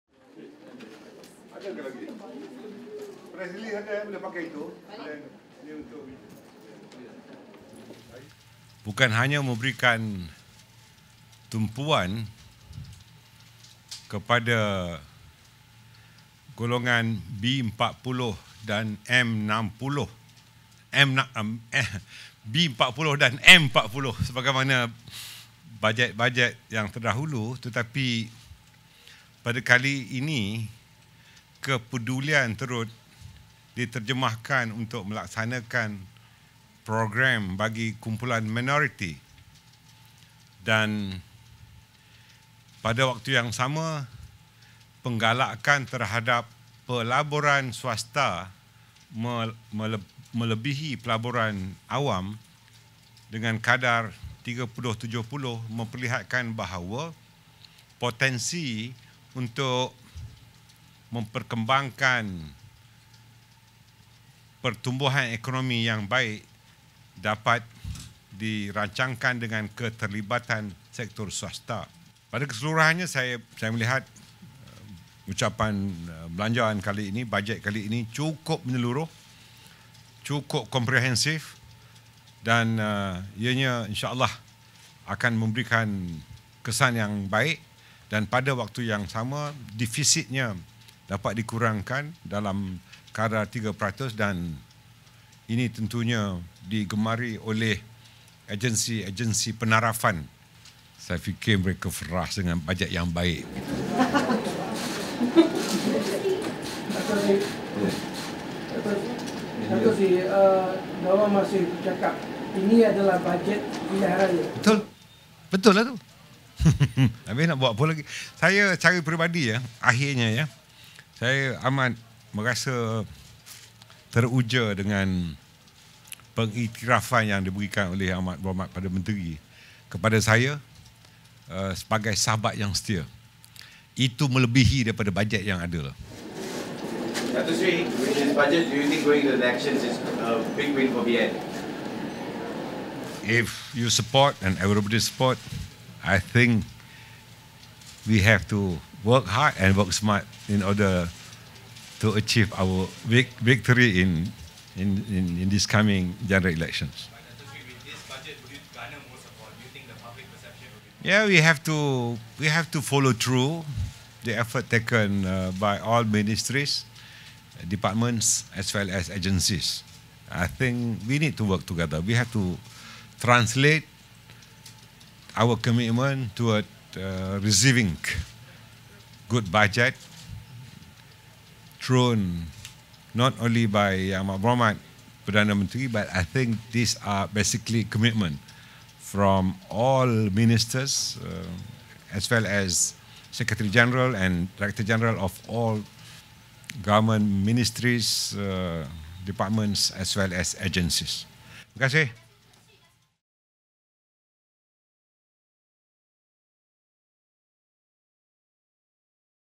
Budget 2018: Yes, it's an election budget, says Zahid
Deputy Prime Minister Datuk Seri Dr Ahmad Zahid Hamidi agreed that Budget 2018, tabled on Friday, is an 'election budget' which oversees all segments of the people and urged all ministries, agencies as well as  departments to translate the federal spending into effective and efficient services for the public.